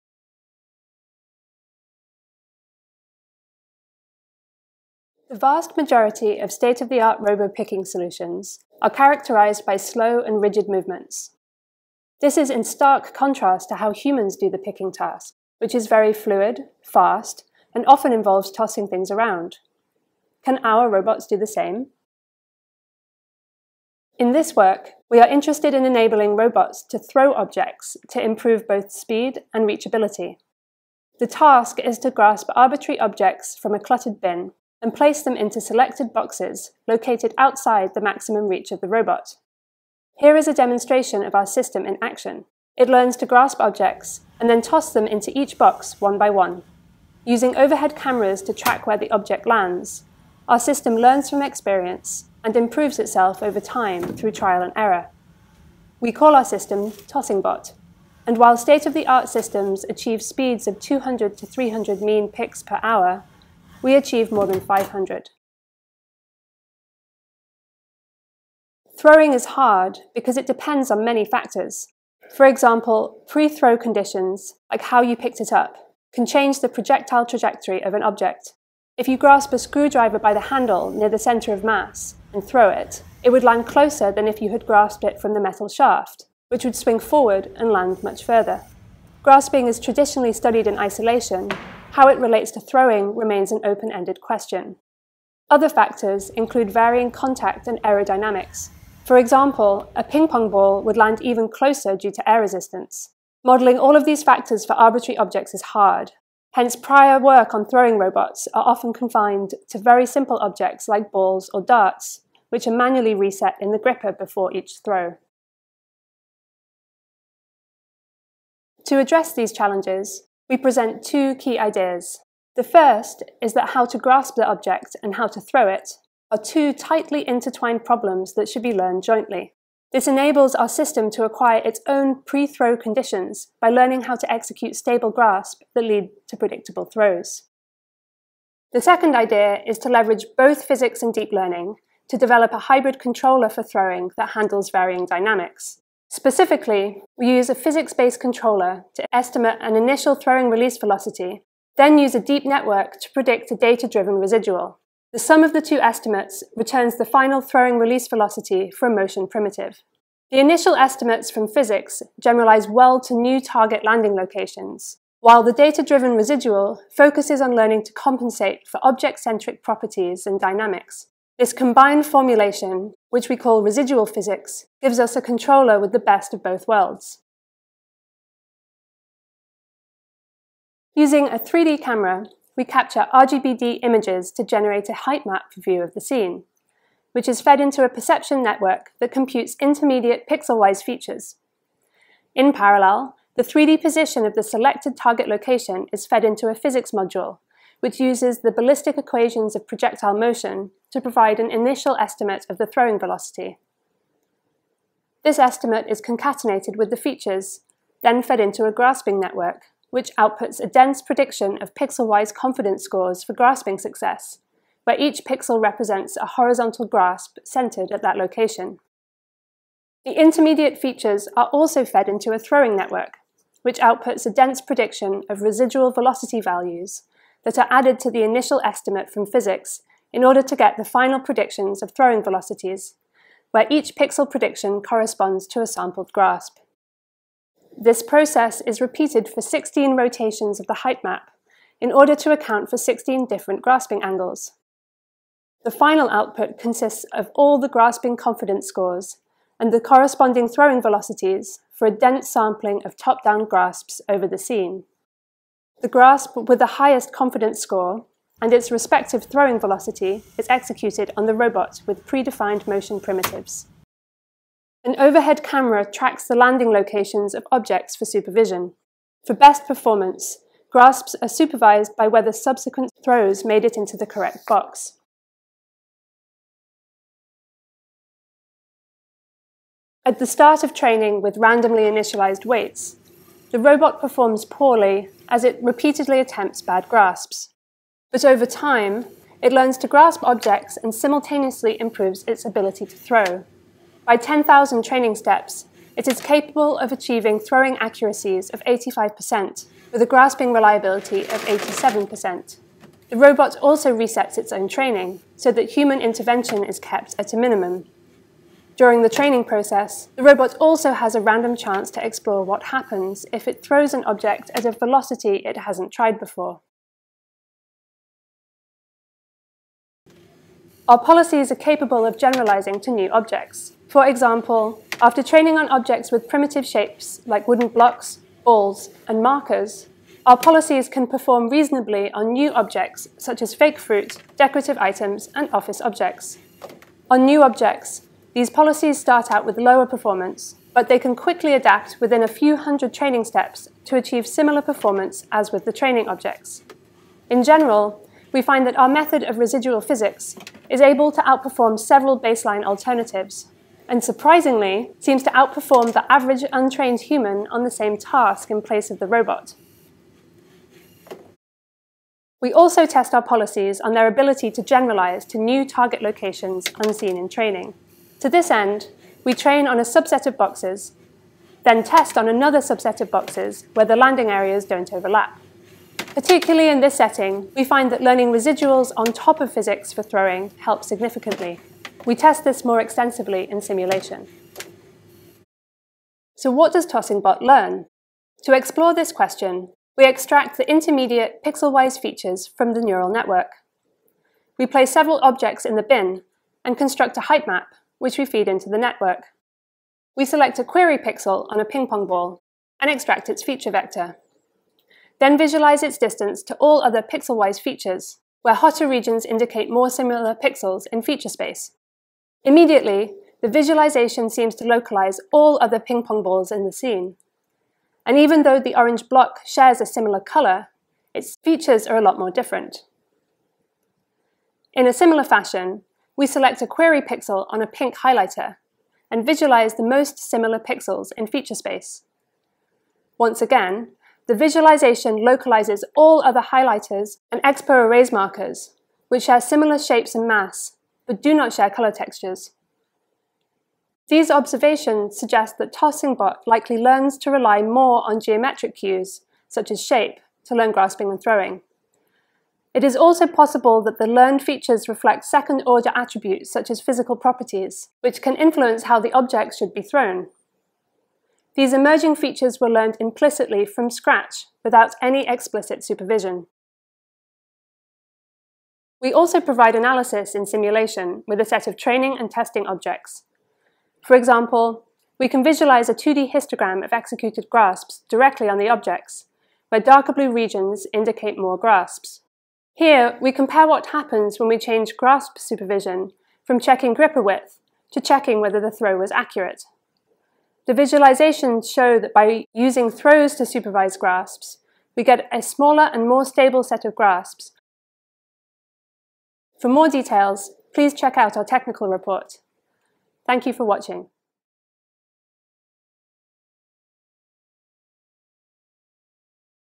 TossingBot: Learning to Throw Arbitrary Objects with Residual Physics
Abstract: Throwing is an excellent means of exploiting dynamics to increase the capabilities of a manipulator. In the case of pick-and-place for example, throwing can enable a robot arm to rapidly place objects into selected boxes outside its maximum kinematic range – improving its physical reachability and picking speed. However, precisely throwing arbitrary objects in unstructured settings presents many challenges: from acquiring reliable pre-throw conditions (e.g. initial pose of object in manipulator) to handling varying object-centric properties (e.g. mass distribution, friction, shape) and dynamics (e.g. aerodynamics). In this work, we propose an end-to-end formulation that jointly learns to infer control parameters for grasping and throwing motion primitives from visual observations (images of arbitrary objects in a bin) through trial and error. Within this formulation, we investigate the synergies between grasping and throwing (i.e., learning grasps that enable more accurate throws) and between simulation and deep learning (i.e., using deep networks to predict residuals on top of control parameters predicted by a physics simulator). The resulting system, TossingBot, is able to grasp and throw arbitrary objects into boxes located outside its maximum reach range at 500+ mean picks per hour (600+ grasps per hour with 85% throwing accuracy); and generalizes to new objects and target locations.

Narration: Laura Graesser